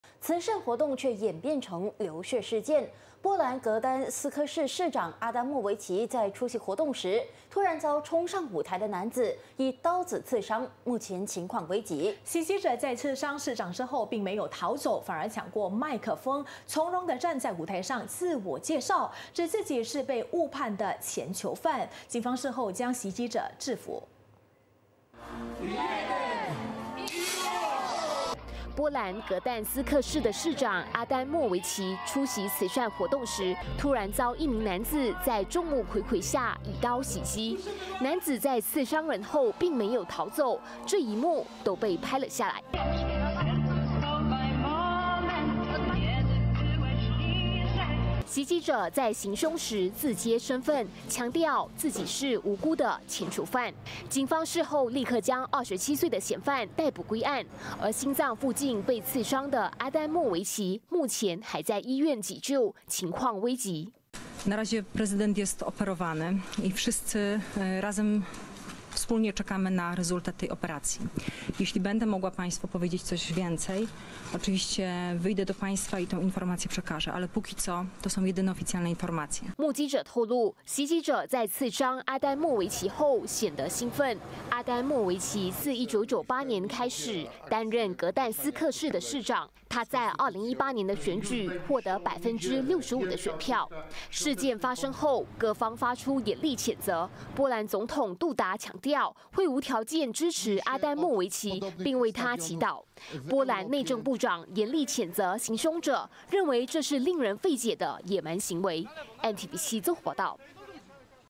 男子众目睽睽行凶 波兰市长伤势严重
慈善活动却演变成流血事件。波兰格但斯克市市长阿丹莫维奇在出席活动时，突然遭冲上舞台的男子以刀子刺伤，目前情况危急。袭击者在刺伤市长后并没有逃走，反而抢过麦克风，从容站在舞台上自我介绍，指自己是被误判的前囚犯。警方事后将袭击者制伏。